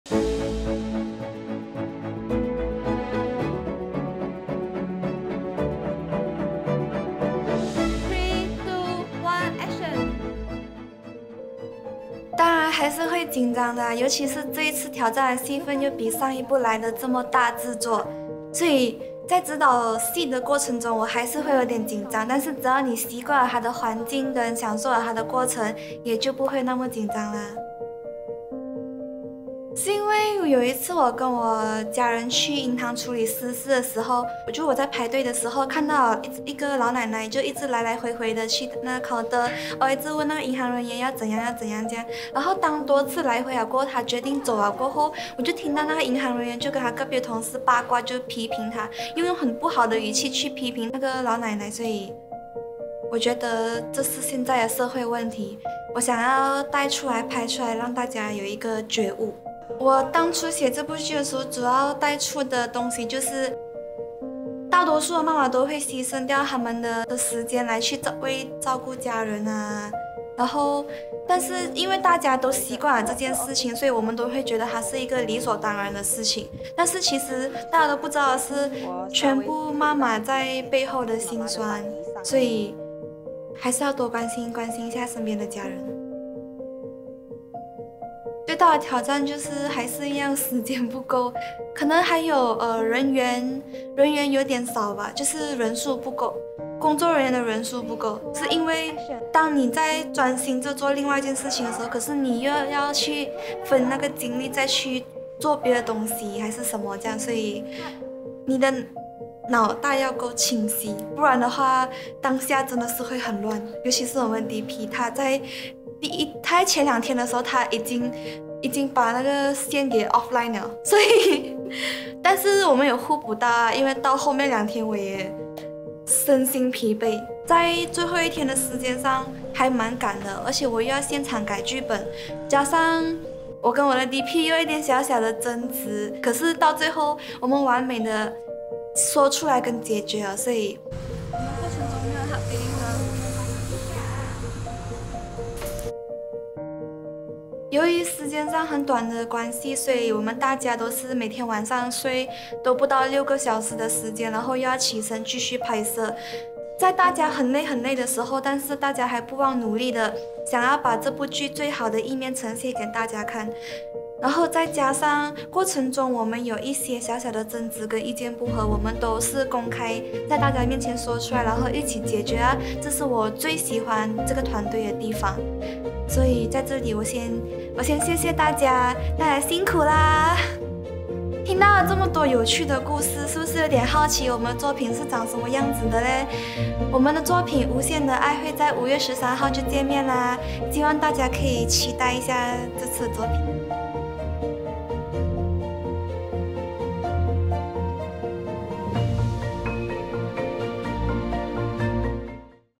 无限的爱 - 幕后花絮 - Part 1
我们是马来西亚的一个影视创意团队，在第一部短片之后，我们很高兴在即将到来的母亲节中推出今年的第二部温馨剧情故事短片。让我们一起观看幕后花絮，并倾听导演凯琦幕后经历分享吧！

We are absolutely thrilled to present our 2nd short film of the year during the upcoming Mothers day, following the achievements of our first one! We invite you to join us in exploring the Behind The scene of our making of the short film and delight in hearing director Kai Qi's insightful thoughts and experiences. Let's celebrate the creative process together!

Title: 无限的爱
Release Date: 13th May 2023
Director 导演: Wong Kai Qi
Director Of Photography / Cinematographer 摄影指导: Cynthia Ong
Sound Mixer 收音: Xin Yi
Gaffer 灯光: Dannis Wong
Art and Props 道具美术: Scarlett Liew
Script supervisor 场记: Kai Wei
Talent Manager: Ellicia Wong
Behind The Scene & Coordination 幕后: Shanghwan

Cast: May Liew, Darren, Elynn, Joey
A Short Film by 8Three

00:00 - 00:11 - Intro
00:11 - 00:29 - Do you still feel nervous/还会紧张吗?
00:29 - 01:26 - The inspiration of this story/故事灵感来源
01:26 - 02:27 - The challenges/最大挑战
02:27 - 03:00 - What to tell the team/想和团队说